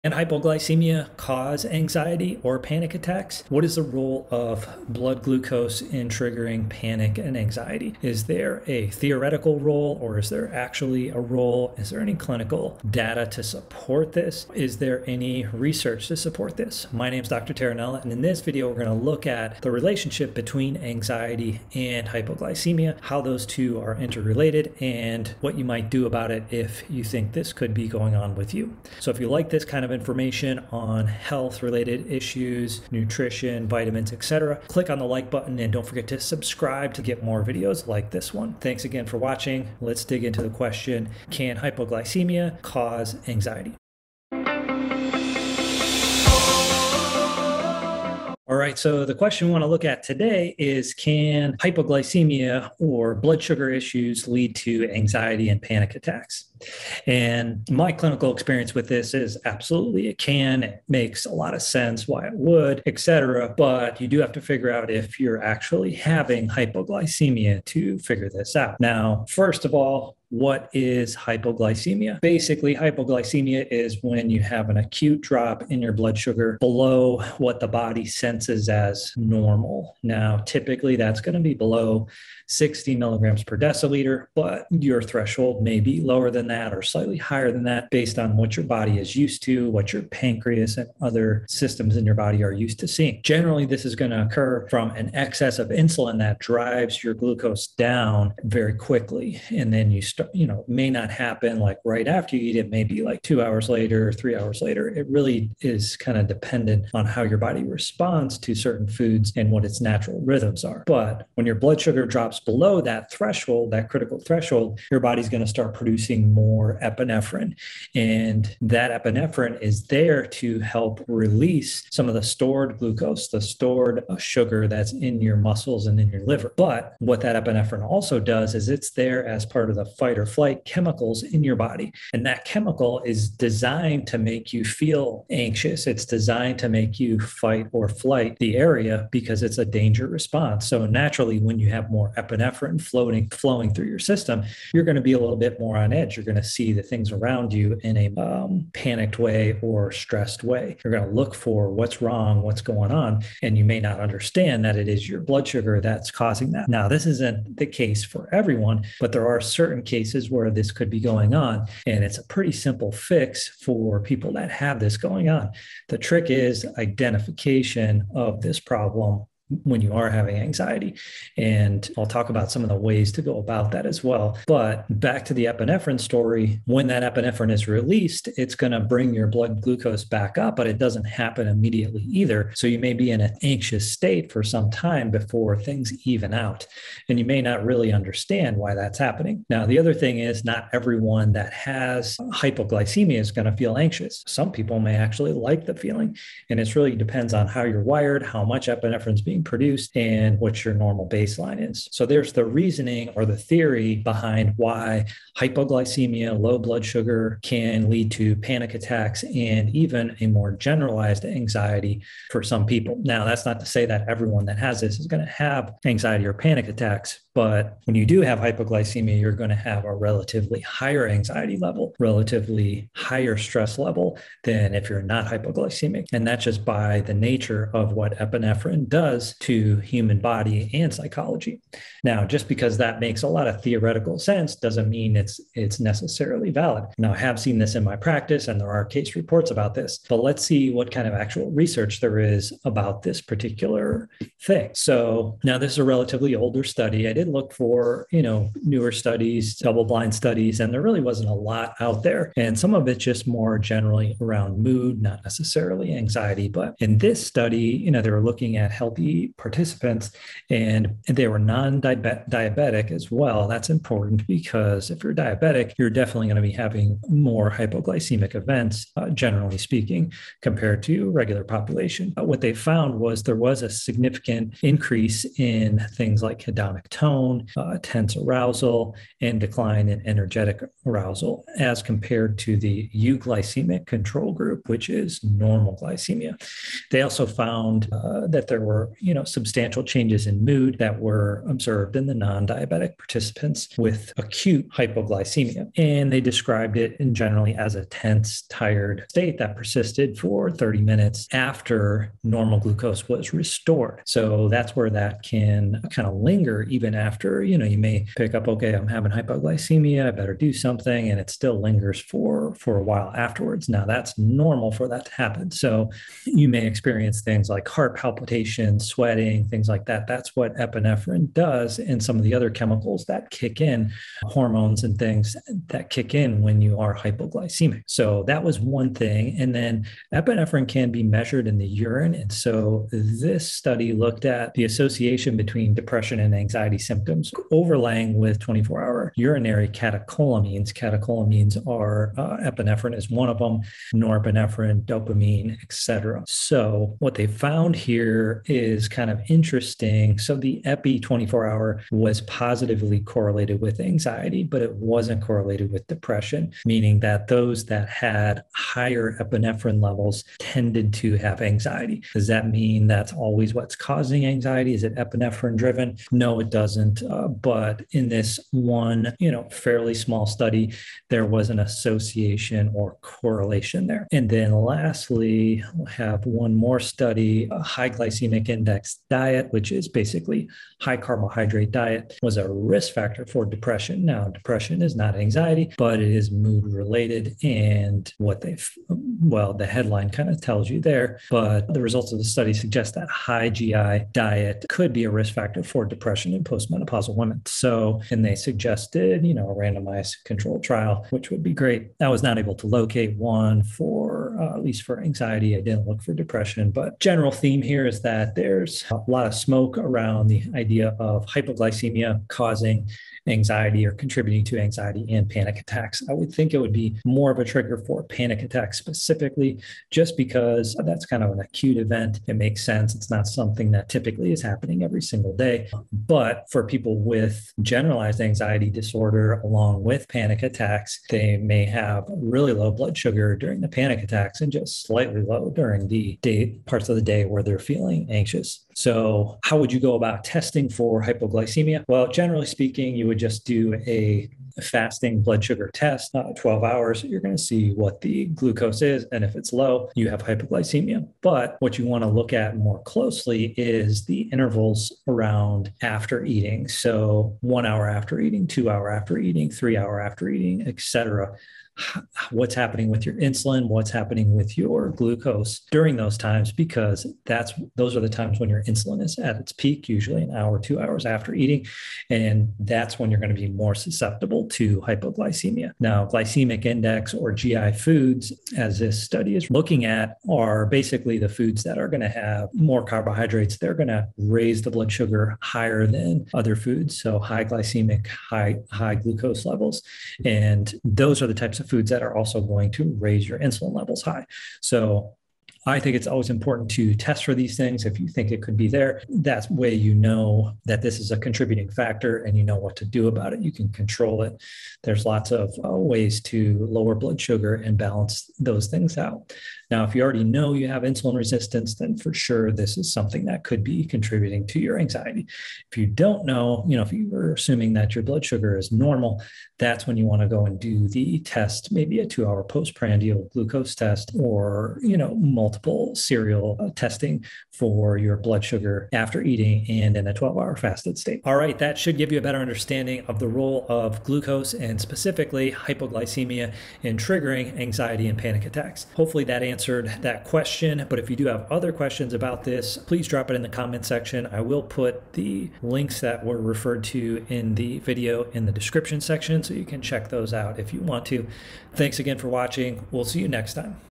Can Hypoglycemia Cause Anxiety and Panic Attacks?
Can hypoglycemia cause anxiety or panic attacks?  What is the role of blood glucose in triggering panic and anxiety?  Is there a theoretical role or is there actually a role?  Is there any clinical data or research to support this.  In this article we look at the relationship between anxiety and hypoglycemia,  how those two are interrelated, and what you might do about it.
The question we want to look at today is can hypoglycemia or blood sugar issues lead to anxiety and panic attacks.   My clinical experience with this is it absolutely can.  It makes a lot of theoretical sense why it would too.  Before you assume this is going on with you, you do have to figure out if you are actually having hypoglycemia.

First of all, what is hypoglycemia?  Hypoglycemia is when you have an acute drop in your blood sugar below what the body senses as normal.  Typically that's going to be below 60 milligrams per deciliter.  However, your threshold may be lower than that or slightly higher than that. This is based on what your body is used to what your pancreas and other tissues in your body are used to.  Generally hypoglycemia comes from an excess of insulin that drives your glucose down very quickly.  Then within 30 minutes to 3 hrs after, your blood sugar drops below that critical threshold.  How quickly the drop occurs is different for different people.  For most it occurs within the first hour.  Once your blood sugar drops, your body starts producing more epinephrine  and cortisol.  Epinephrine and cortisol are there to help release some of the stored glucose and the stored sugar that's in your muscles and in your liver.   What that epinephrine also does is active the fight-or-flight chemicals in your body. That chemical, epinephrine is designed to make you feel anxious.  It is designed to make you fight or flight the area.  It is a danger response.

Naturally when you have more epinephrine flowing through your system, you will be a little bit more on edge and see the things around you in a panicked way or stressed way.  That panicked feeling will cause you to look for what's wrong.  You may not understand that it is your blood sugar that's causing this in the moment.  Keep in mind that not everyone has hypoglycemia and even those that do may have anxiety in addition to hypoglycemia.  Still there are certain cases where this could be the main thing going on.  Hypoglycemia is not that hard to  fix for people.   The trick is identification of the problem.  First a little more on the epinephrine story.   When that epinephrine is released it's going to bring your blood glucose back up but it doesn't happen immediately.   You may be in an anxious state for some time before things even out and you may not really understand why that is happening. The other thing is not everyone that has hypoglycemia is going to feel anxious.  Some people may actually like the feeling.  It really depends on how you're wired, how much epinephrine is being produced and what your normal baseline is.  That's the reasoning or the theory behind why hypoglycemia and low blood sugar can lead to panic attacks and generalized anxiety for some people.   That's not to say that everyone that has this is going to have anxiety or panic attacks.  When you do have hypoglycemia, you're going to have a relatively higher anxiety level, and relatively higher stress level than if you're not hypoglycemic.  That is just by the nature of what epinephrine does to human body and psychology.  Just because this makes a lot of theoretical sense, doesn't mean it is necessarily valid.

Research Behind Can Hypoglycemia Cause Anxiety?

I have seen this in my practice and there are case reports about this. However, let's see what kind of actual research there is about this.  A relatively older study looked at this question in healthy (non-diabetic) participants.   With diabetics you will definitely have more hypoglycemic events compared to the regular population.  What the researchers found was there was a significant increase in things like hedonic tone, tense arousal, and a decline in energetic arousal compared to the normal glycemic control group.  They also found that there were substantial changes in mood observed in the healthy participants with acute hypoglycemia.  The participants described it generally as a tense tired state that persisted for thirty minutes after normal glucose was restored.  That's where that feeling can linger even after your blood sugar normalizes.

You may start having hypoglycemia and think, I better do something about it.  Yet it may linger for for a while afterwards and that's normal.  Some people experience things like heart palpitations, sweating, anxiety and things like that.  This is what epinephrine and some of the other chemicals that kick in do when you are hypoglycemic.